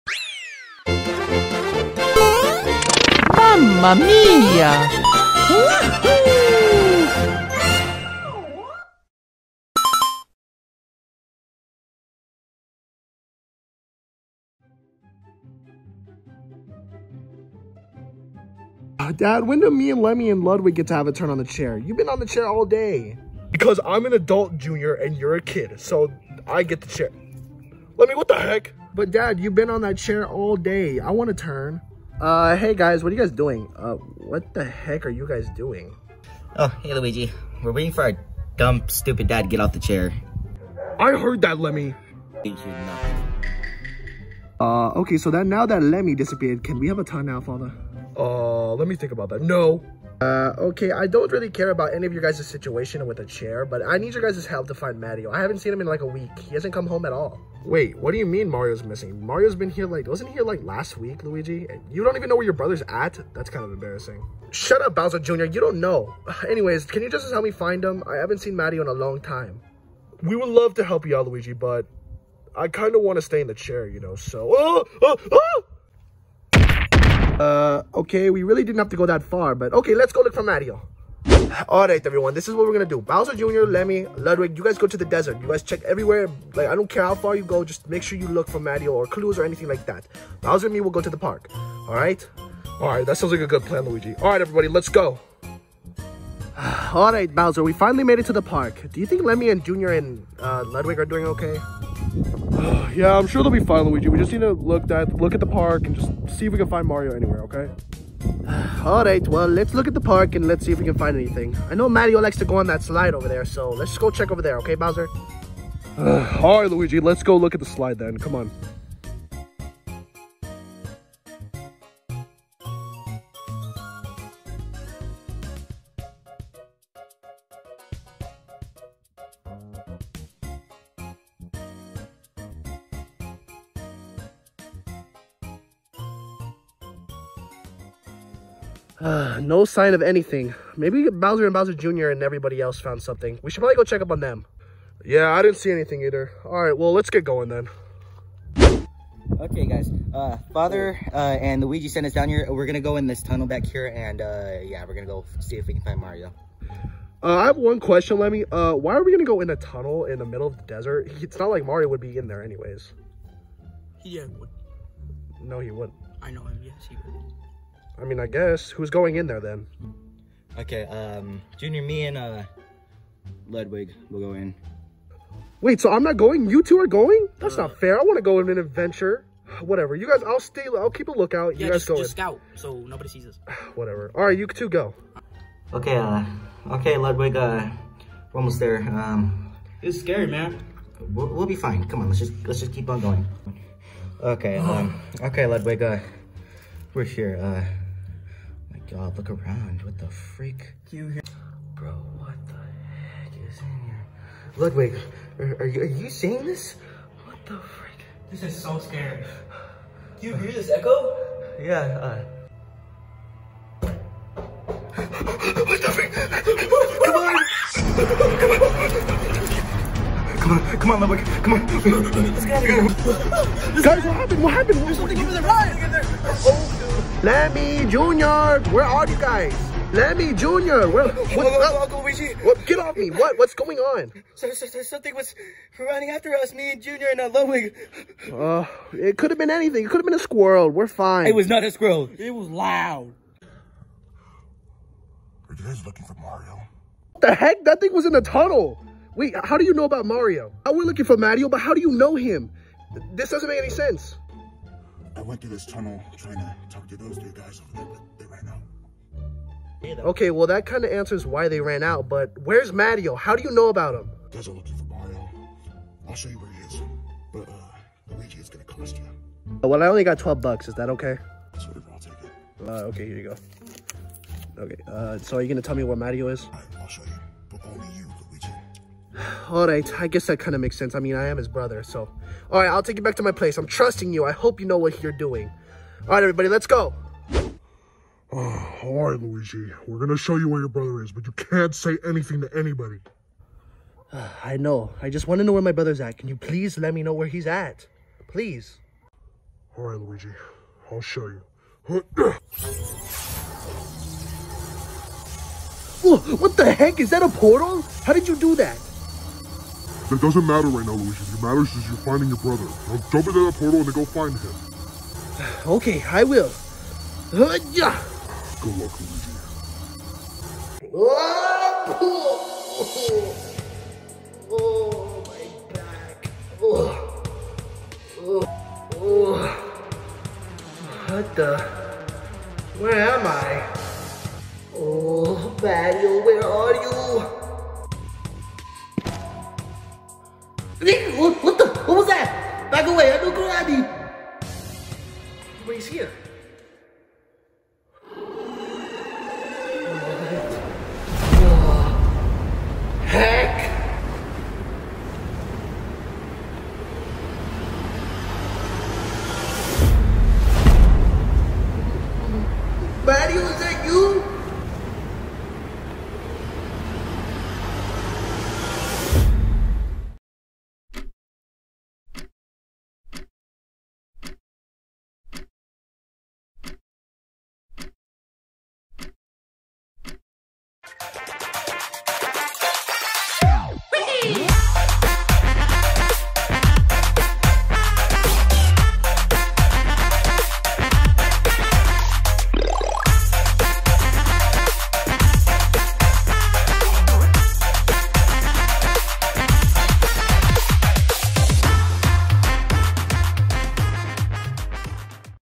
SMDBROS: Mario missing?!?!
Mario is missing where can he be?

                                                   Game links
Fortnite account: marioaddictionz

Follow my xbox account: marioaddictionz